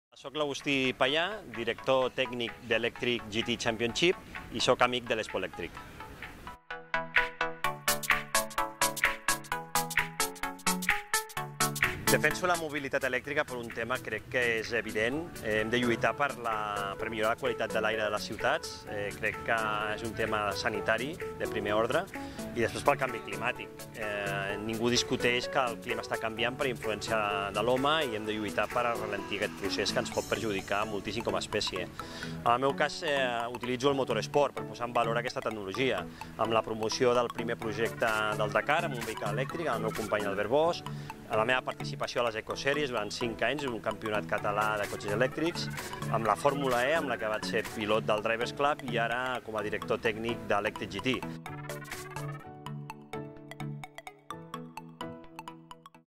Amigos de Expoelectric - Agustí Payá, director técnico de Electric GT Championship
Expoelectric ha impulsado la iniciativa Amigos de Expoelectric con la voluntad de crear una red de embajadores para promover la movilidad eléctrica y sensibilizar y familiarizar a la ciudadanía con los vehículos de cero emisiones y eléctricos.

Los Amigos de Expoelectric son personalidades comprometidas con el medio ambiente y con la sostenibilidad, que dan soporte al vehículo eléctrico, nos explican sus beneficios y nos invitan a sumarnos también a la revolución que supone la movilidad eléctrica.